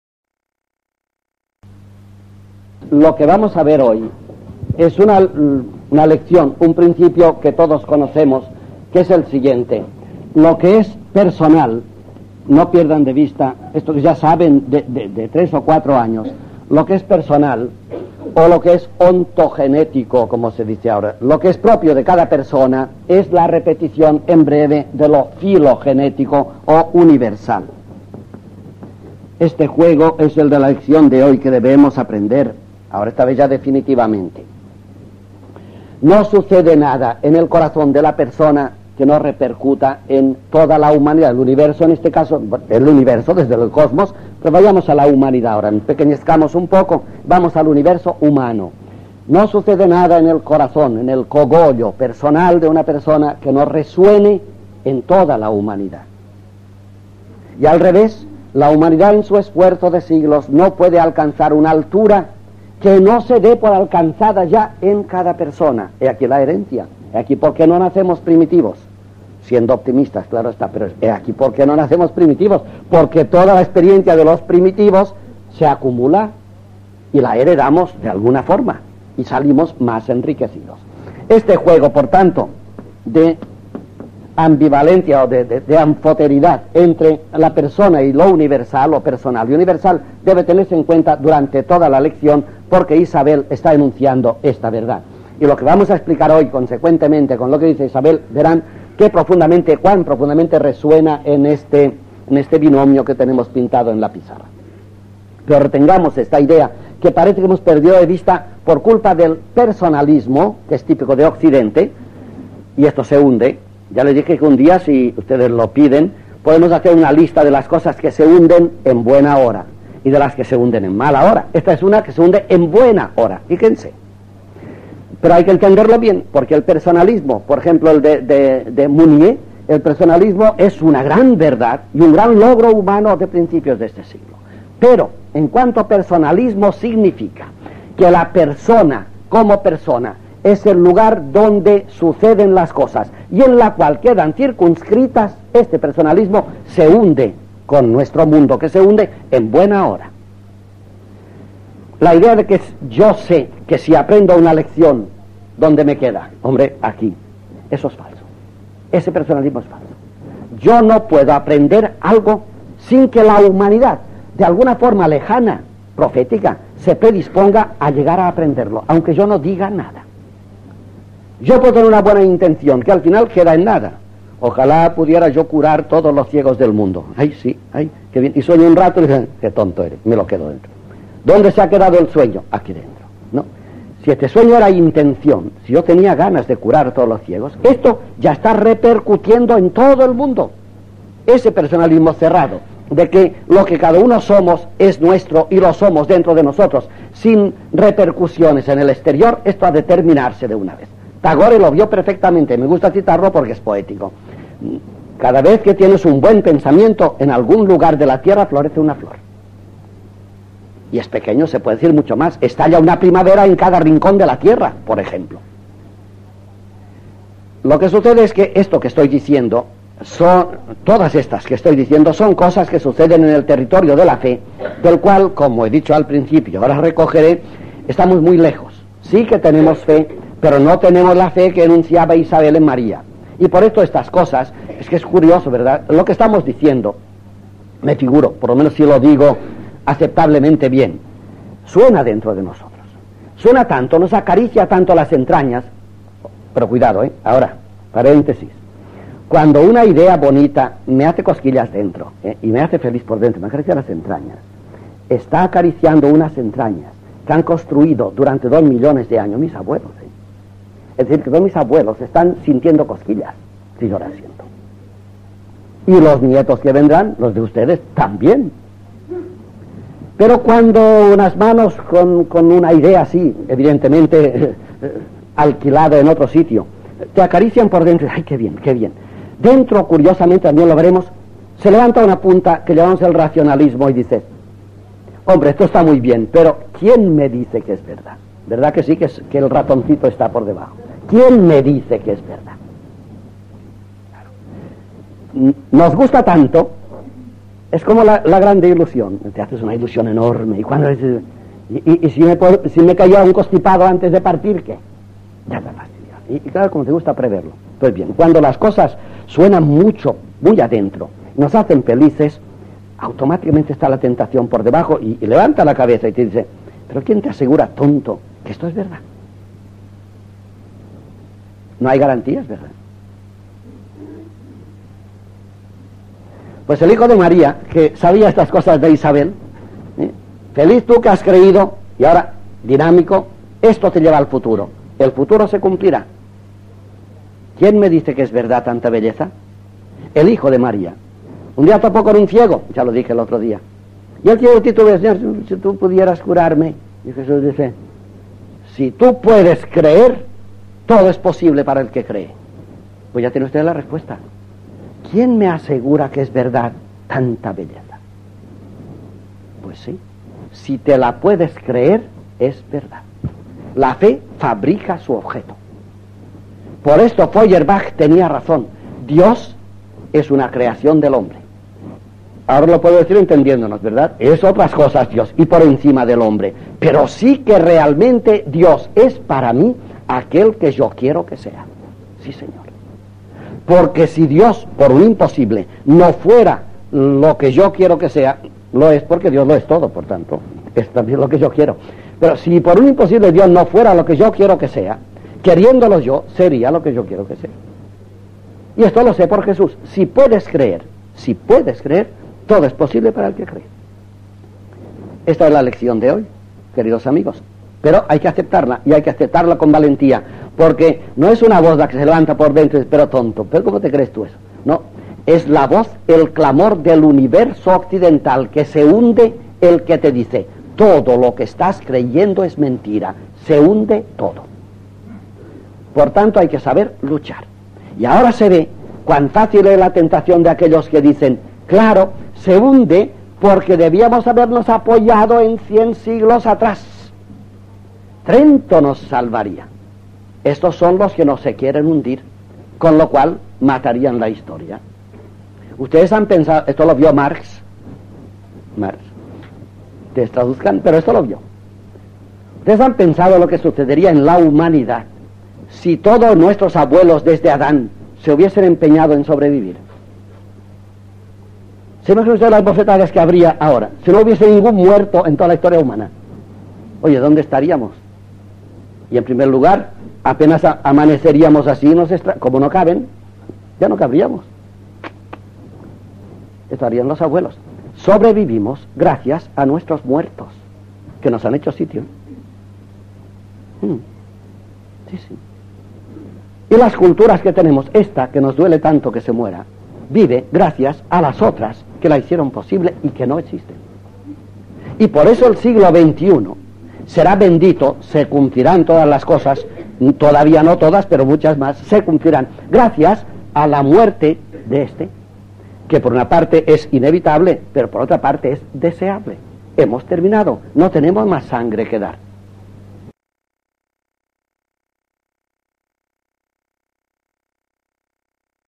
El hombre y su destino (La solución está en manos del Hombre).L.4/10 Cap.1/7Curso P. Antonio Oliver
Lección 4: La solución está en manos del Hombre [11/Ene/92]
1 Feliz porque has creído

De todos los males que nos aquejan la solución ya está aquí: en manos del Hombre

1 Feliz porque has creído
2 Todo es posible
3 Fíate
4 y no mires atrás
5 Conseguirás lo imposible
6 Los Magos llegaron buscando
7 y se volvieron llenos